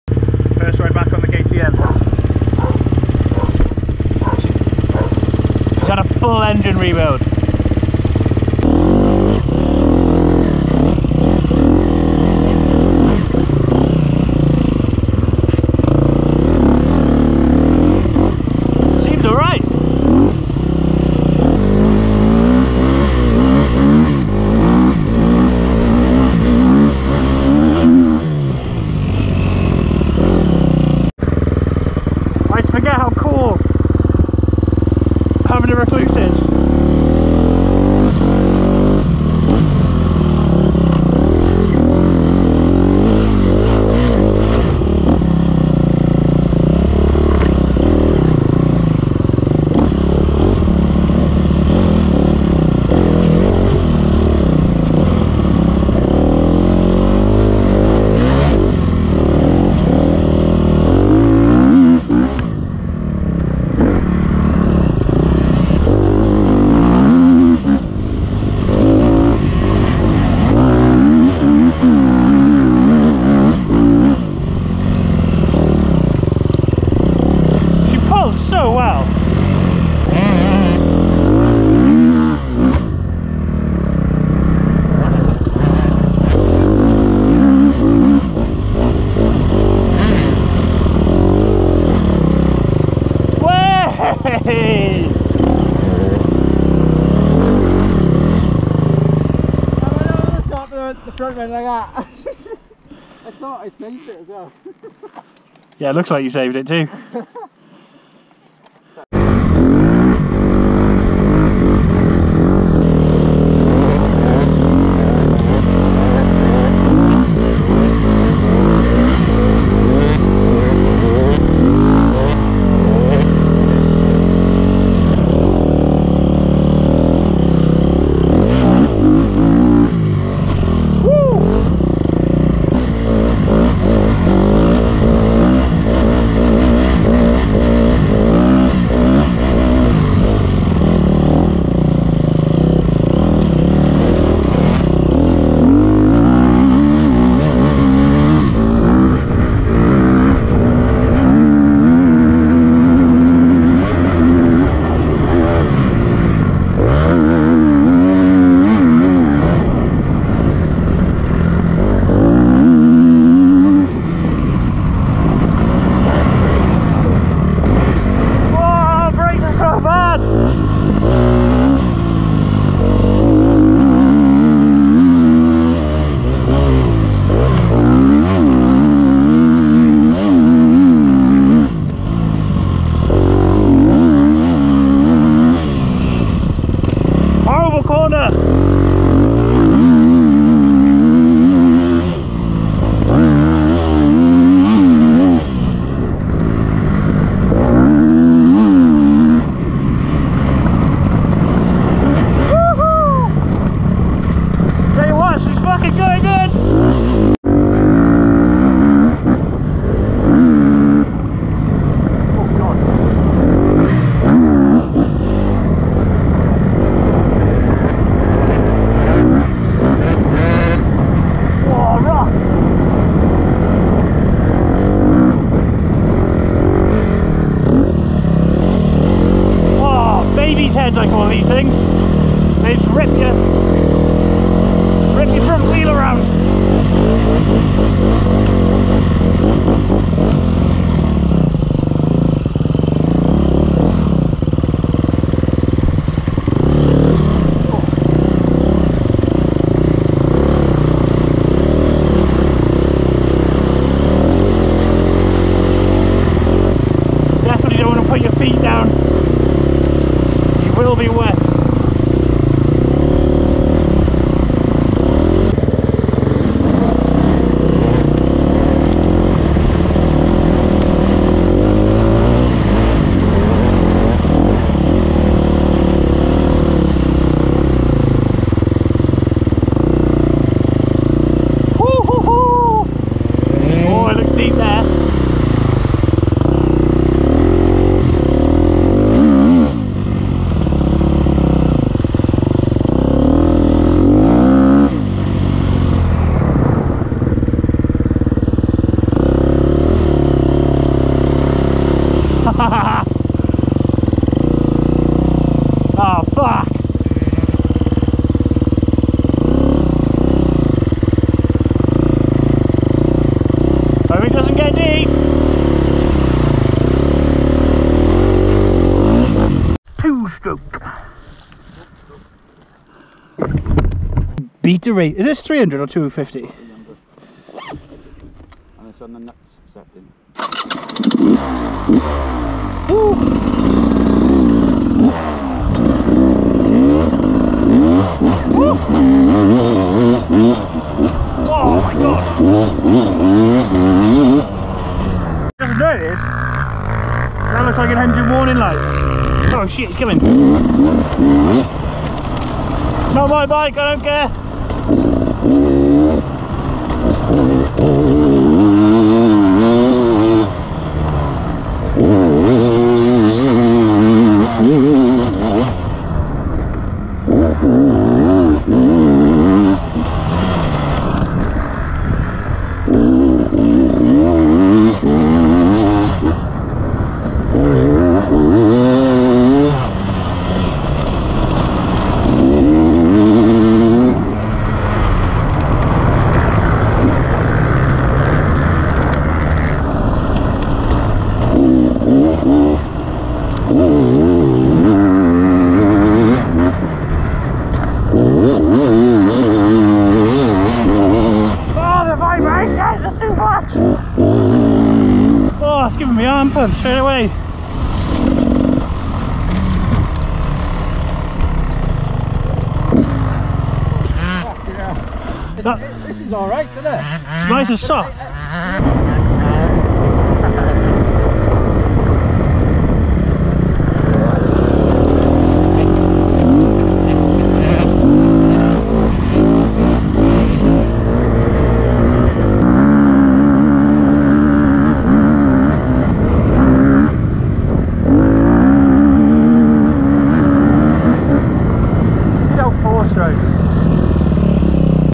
First ride back on the KTM XCF 250f After a full rebuild
Fully rebuild the 2012 KTM xcf 250. It took some doing as it turns out the xcf 2012 is its own engine. The xcf shares a big part of the sxf 250 from years 07-11 & 13-15 but found out the the hard way the 2012 has its own parts making it a royal pain in the ass to get parts for. I ended up grinding a 2013sxf crank pin down by .05mm each end to fit 🤘🤘 The shortcut that worked